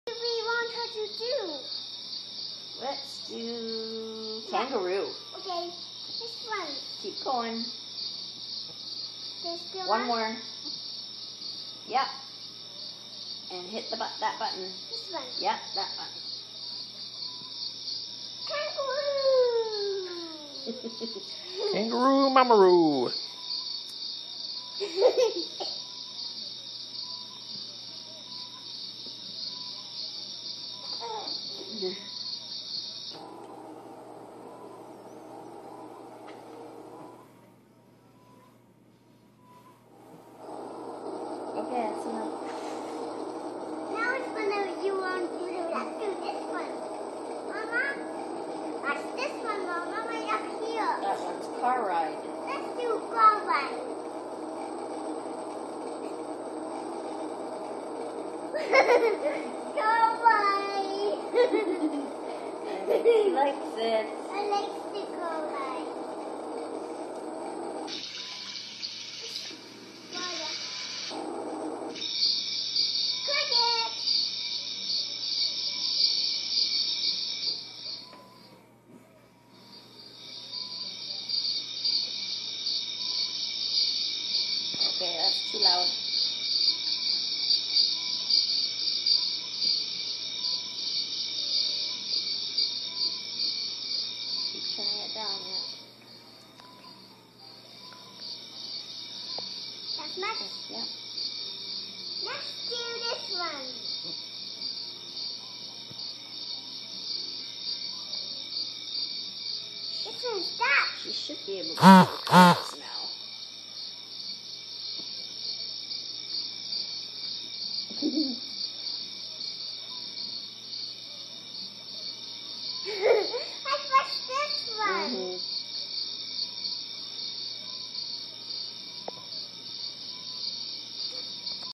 Do you mamaRoo?
Emily and Jax demonstrate the mamaRoo

Thanks go to my coworkers for this crazy contraption, they pitched in for a generous baby shower gift card